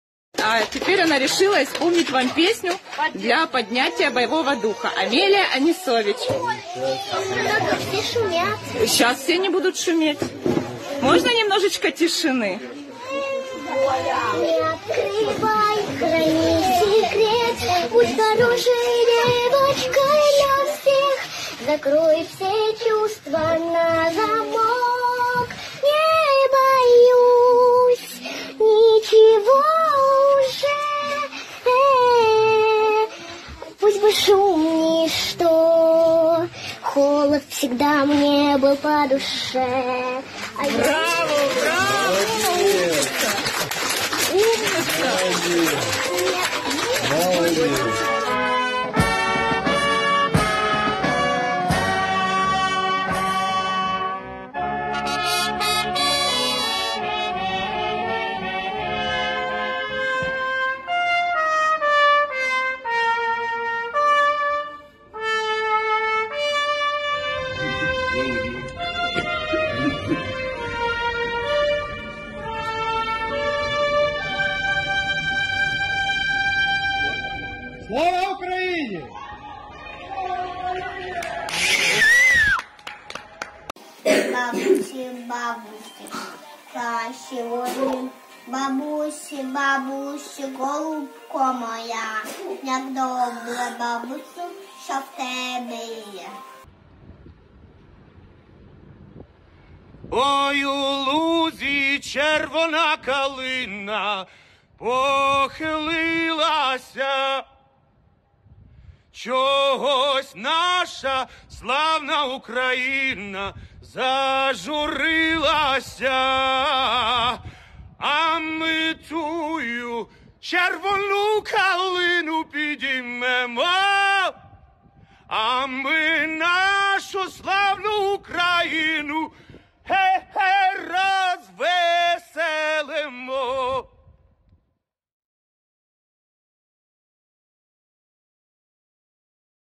Këngë në vendstrehime: Muzika frymëzon Ukrainën e dërrmuar nga lufta
Këngëtarë dhe muzikantë, të rinj e të moshuar, janë incizuar duke luajtur, teksa përpiqeshin të ngrinin moralin e ukrainasve në vendstrehime për mbrojtje nga bombardimet, në metronë e Kievit, dhe në zona të tjera të rrezikuara nëpër vend, ndërsa lufta e Rusisë në Ukrainë po vazhdon.